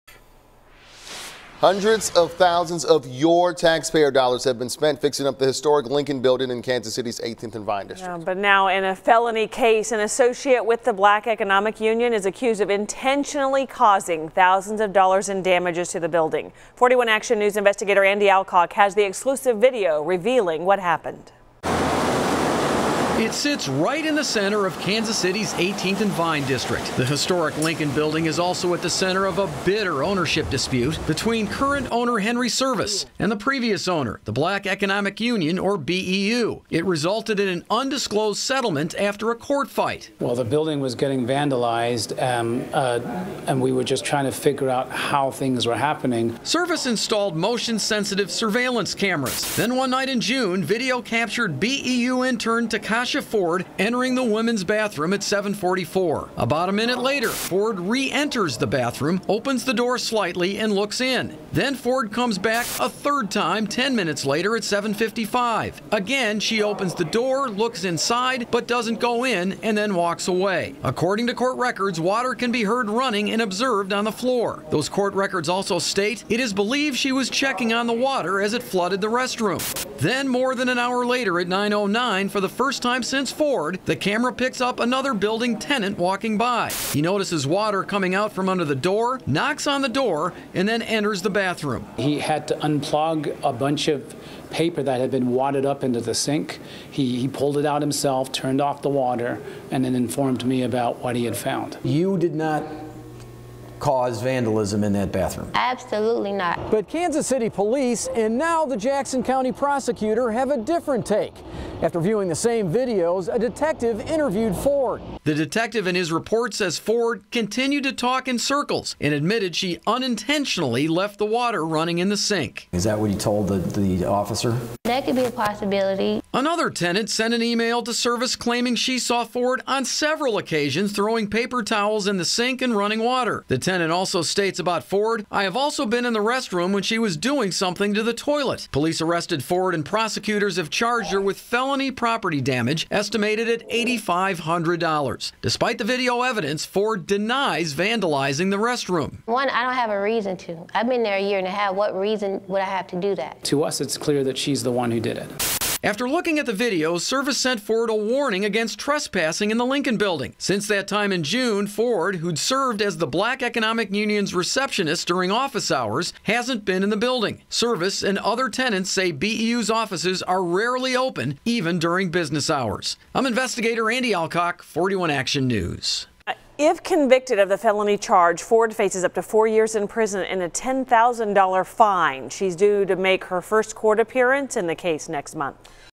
Black Economic Union employee accused of vandalizing Lincoln Building
A Black Economic Union intern is accused of vandalizing the historic Lincoln Building in the 18th and Vine District. Takishia Ford, 39, faces a felony property damage charge after she was accused of deliberately flooding a bathroom in the building in June.